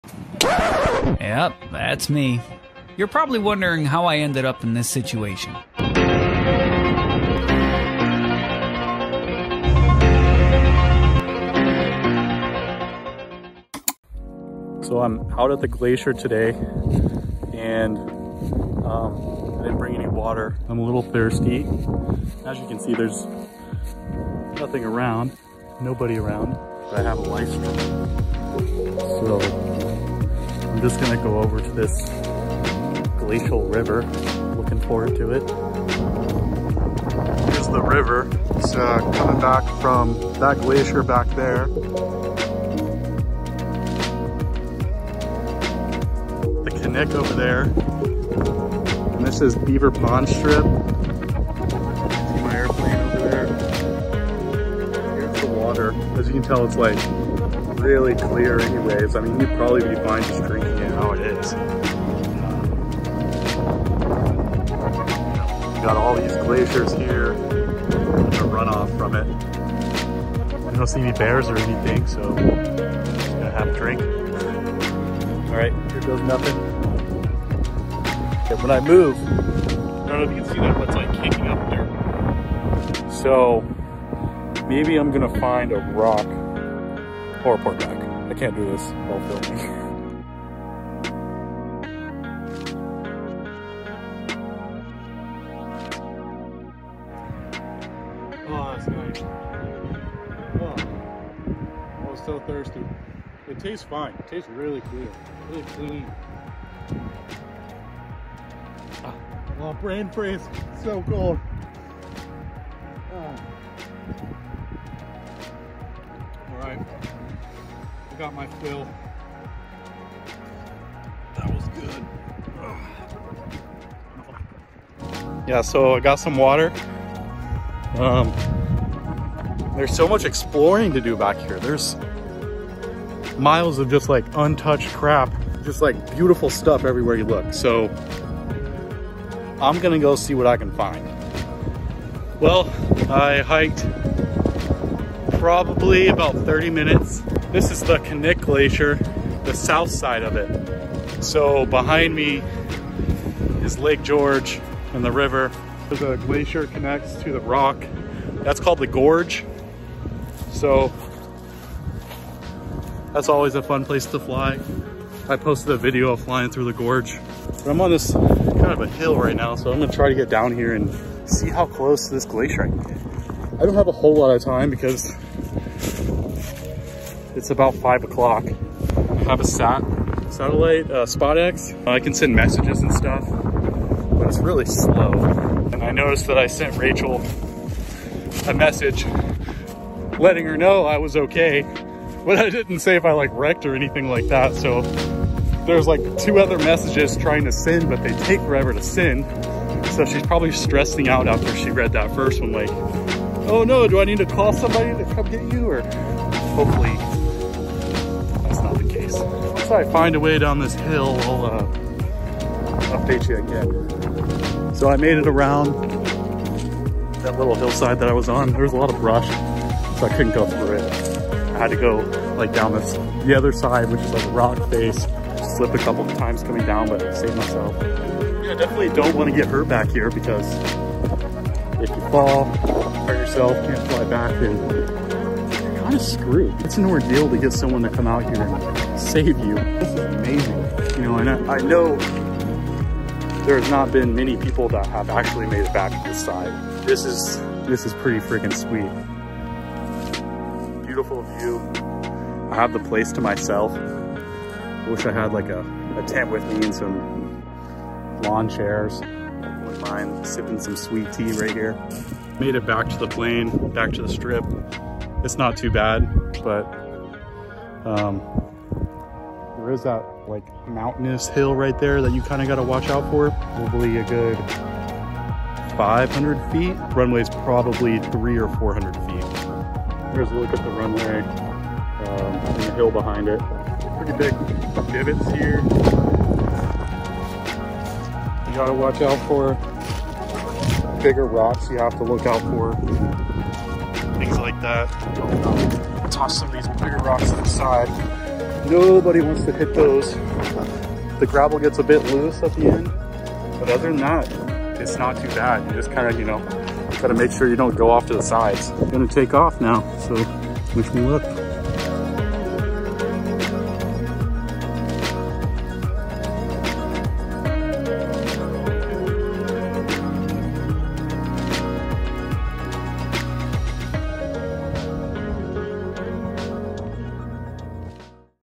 Bush Pilot Problems
After a stop off at the Picnic Table Strip, I decided to give a new harder to reach strip a try. This was my first time landing at the Beaverpond Strip. I finally had a good opportunity to try out my life straw, so I thought I’d do a little VLOG about my experience. Thanks to Rachael for editing this video!! It turned out really well and so I might be doing more like this in the future.